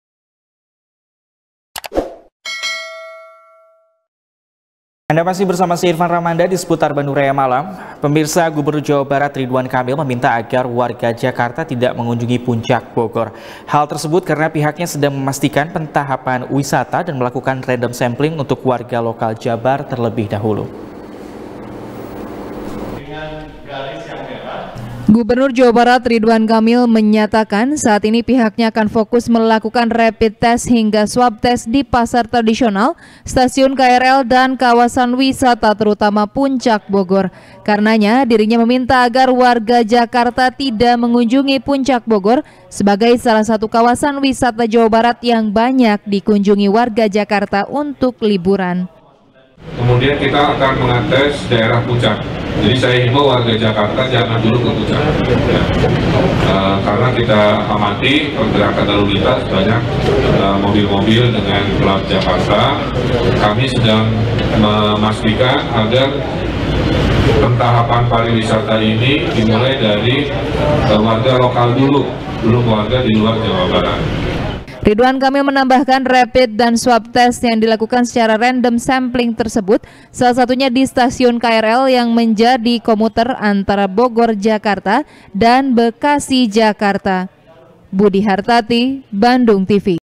Ridwan Kamil Minta Warga Jakarta Tak Ke Puncak Bogor | SBR | BANDUNGTV
SBR | BANDUNGTV | IRFAN RAMANDA | Gubernur Jawa Barat Ridwan Kamil, Meminta Agar Warga Jakarta Tidak Mengunjungi Puncak Bogor. Hal Tersebut Karena Pihaknya Sedang Memastikan Pentahapan Wisata Dan Melakukan Random Sampling, Untuk Warga Lokal Jabar Terlebih Dahulu.